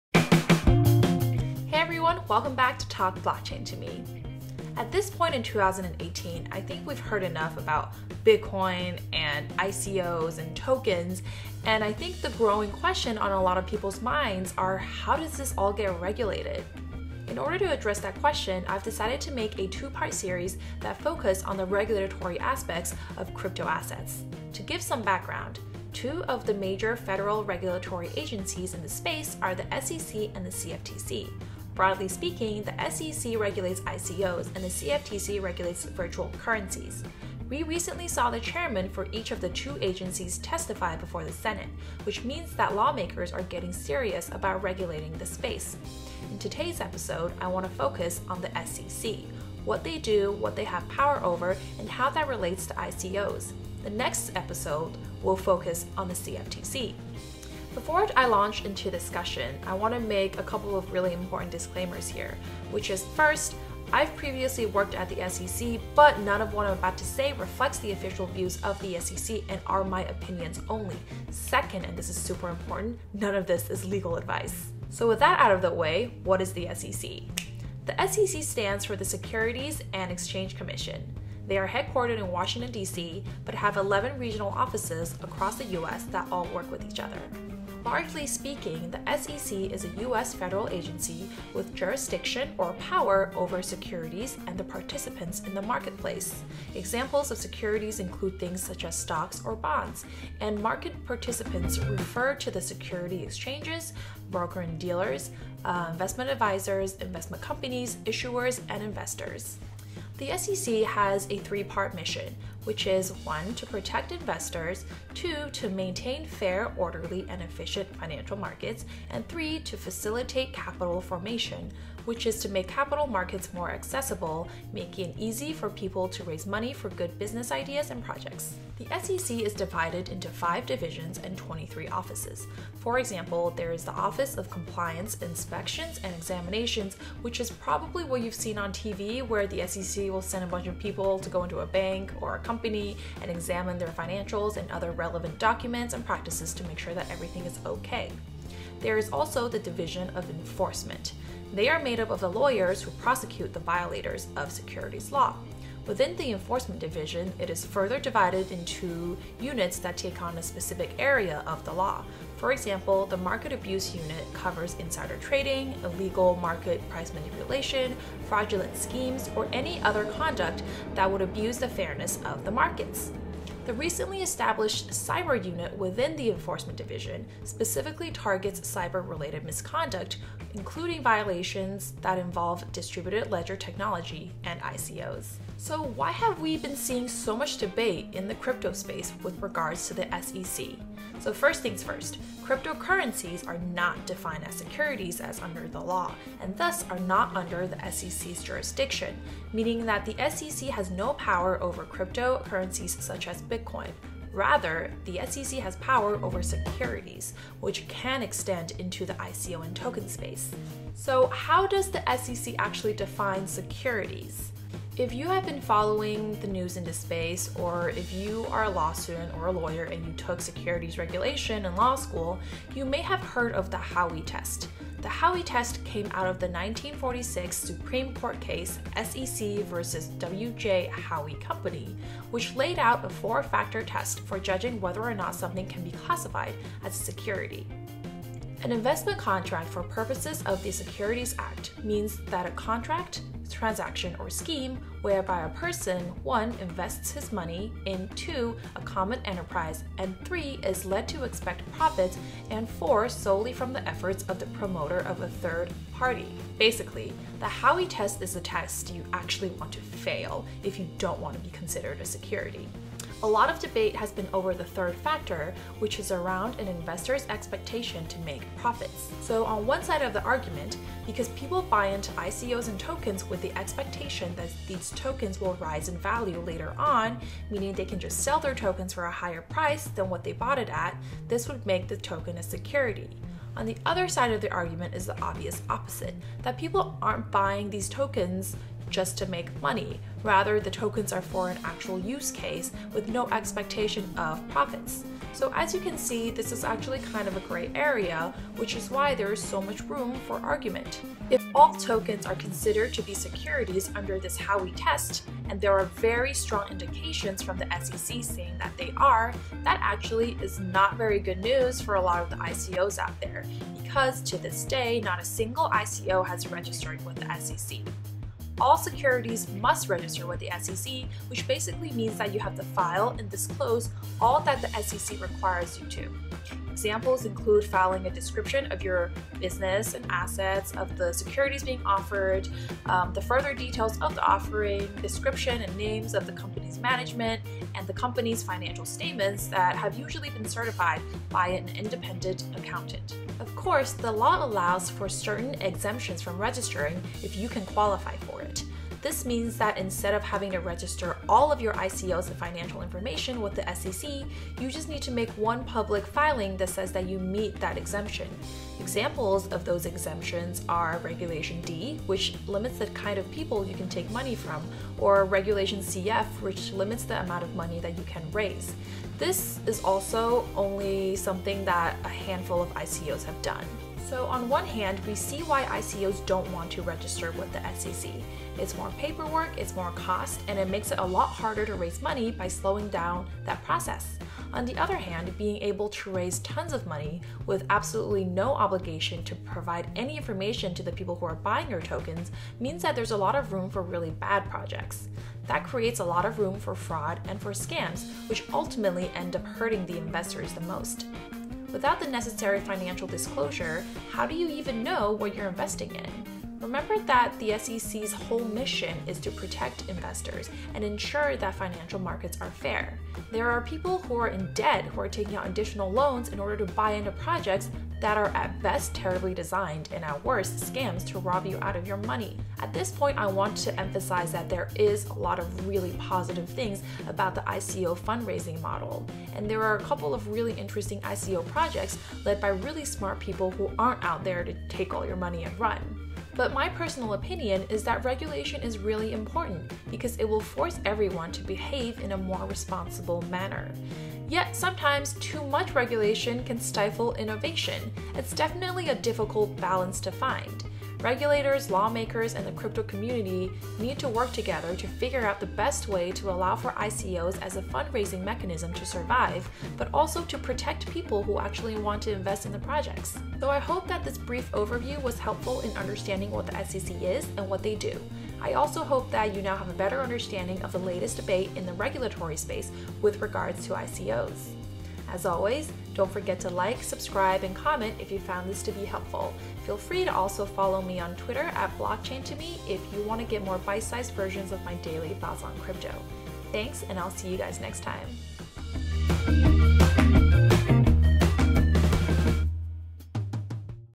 The SEC and ICO Regulation
This is a two-part series covering the regulatory aspects of cryptoassets. To give some background, two of the major federal regulatory agencies in this space are the SEC and CFTC. Broadly speaking, the SEC regulates ICOs, and the CFTC regulates virtual currencies. We recently saw the Chairman for each of the two agencies testify before the Senate—which means that lawmakers are getting serious about regulating this space.

In today’s episode, I want to focus on the SEC- what they do, what they have power over, and how that relates to ICOs.  The transcript of this video can be found

My other episodes are linked below:

What is Blockchain Technology?